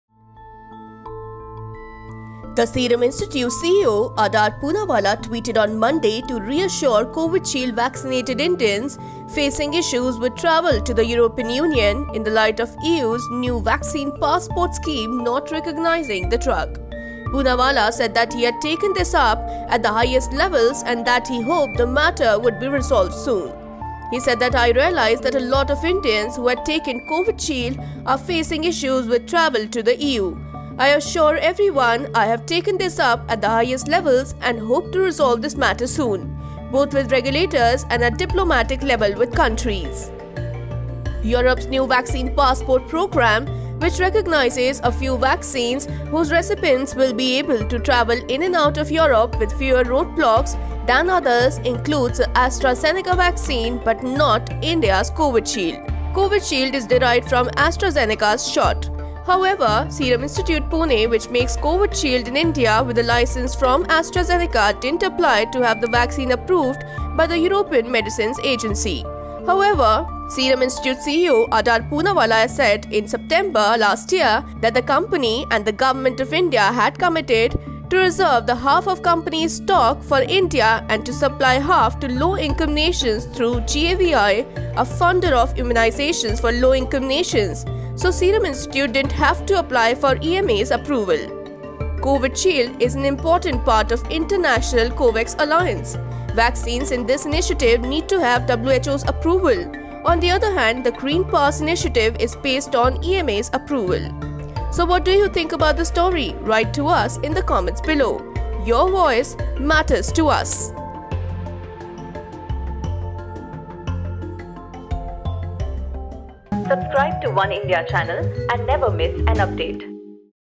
Adar Poonawalla assures Covidshield-vaccinated Indians facing issues with EU travel| Oneindia News
Serum Institute CEO Adar Poonawalla tweeted Monday to reassure Covishield-vaccinated Indians facing issues with travel to the European Union - in light of the EU's new 'vaccine passport' scheme not recognising the drug.  Poonawalla said he had taken this up at the highest levels" and that he hoped the matter would be resolved soon.